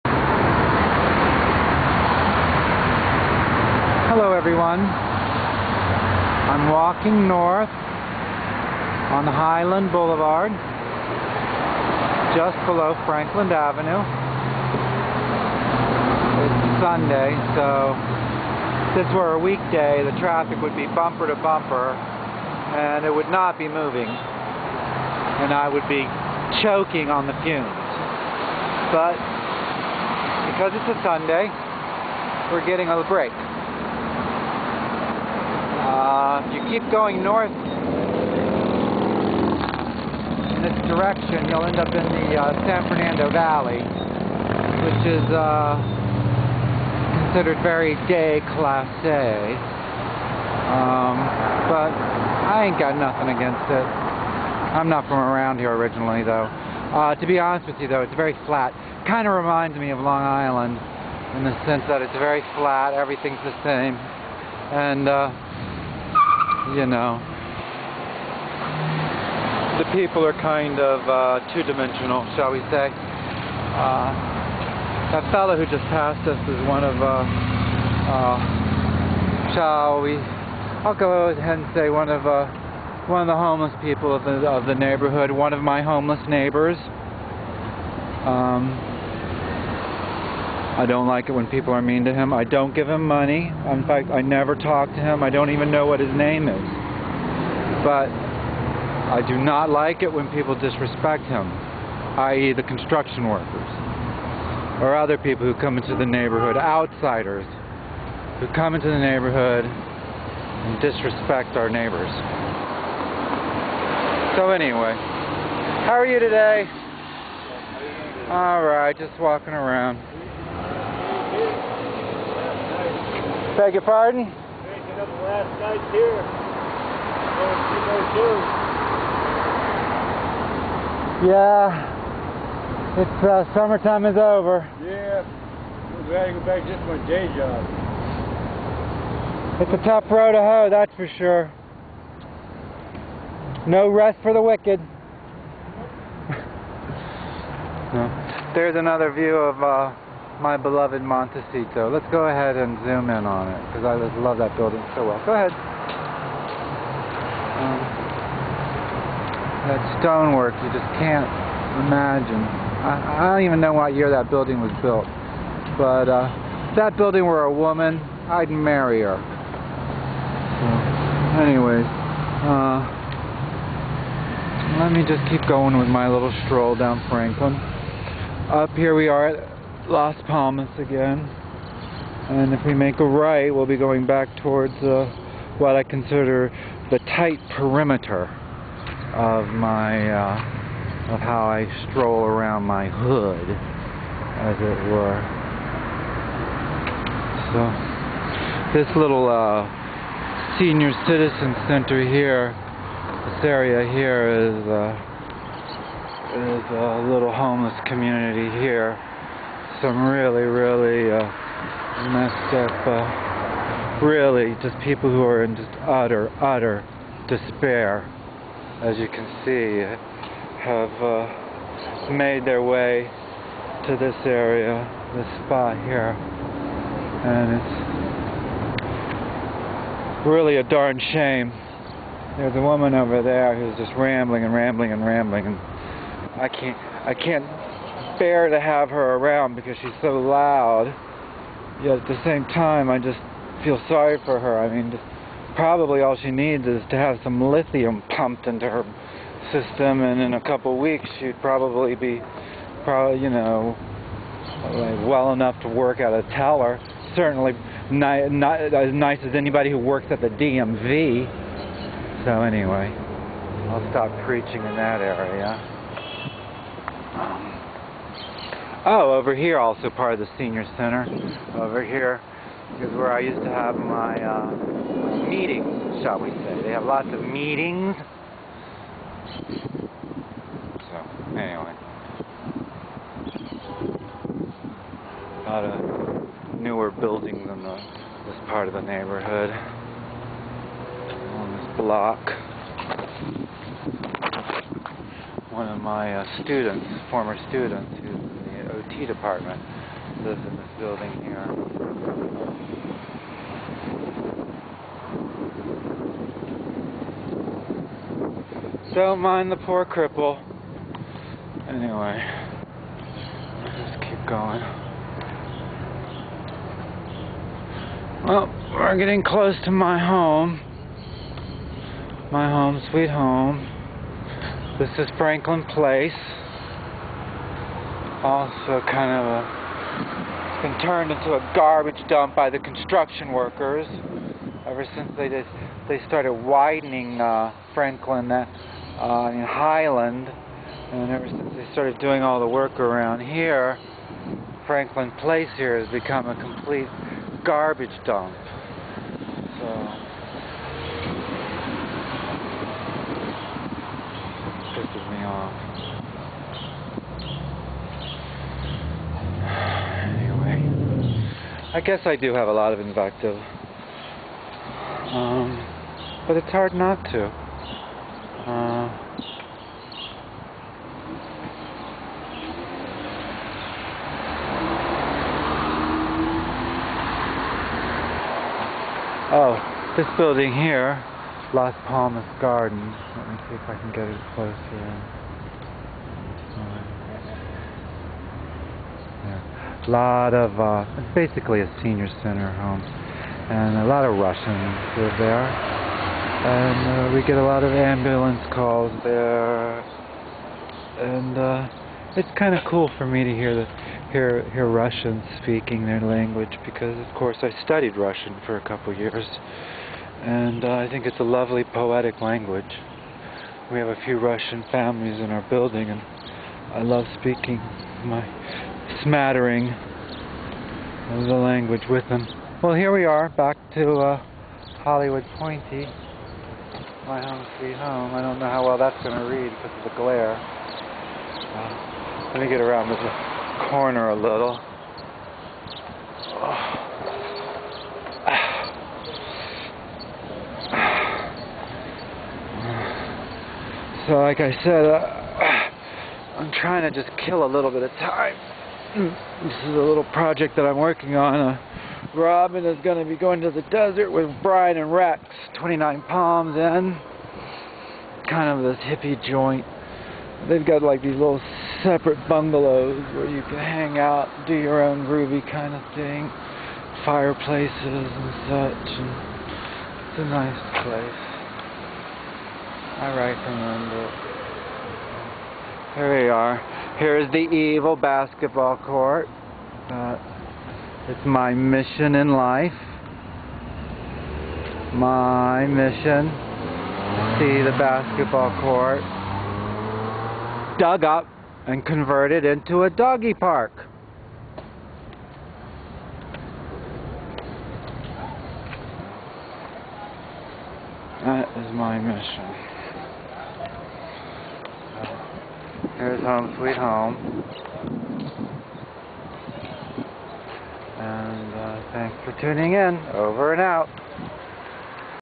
Perambulate
Walk around the block with me in my neiborhood- Yucca Street Hollywood!